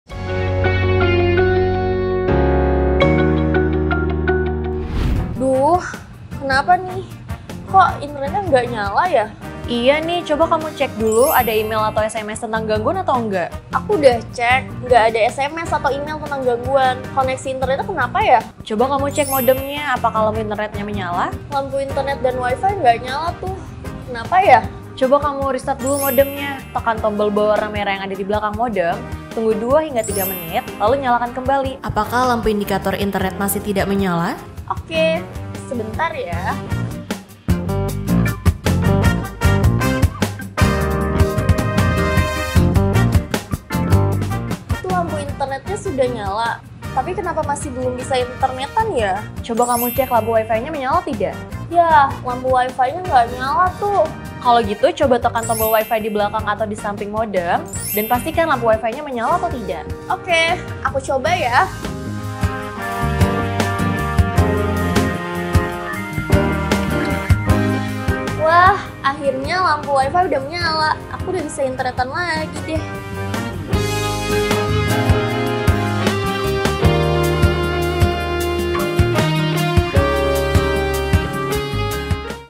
Lampu Internet Di Modem Tidak Menyala
Ikuti cara berikut ini, ya, Sobat Play~
• Pastikan lampu internet menyala.
• Restart modem selama 2-3 menit.
• Setelah lampu internet menyala, pastikan lampu wifi juga sudah menyala sebelum menggunakan jaringan internet melalui gadget atau handphone.
• Jika lampu internet masih belum menyala, silakan menghubungi WA Interaktif MNC Play di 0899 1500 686

⁣⁣⁣⁣𝐌𝐍𝐂 𝐏𝐥𝐚𝐲, 𝐌𝐚𝐢𝐧𝐤𝐚𝐧 𝐃𝐮𝐧𝐢𝐚𝐦𝐮 𝐓𝐚𝐧𝐩𝐚 𝐁𝐚𝐭𝐚𝐬!⁣⁣⁣⁣⁣⁣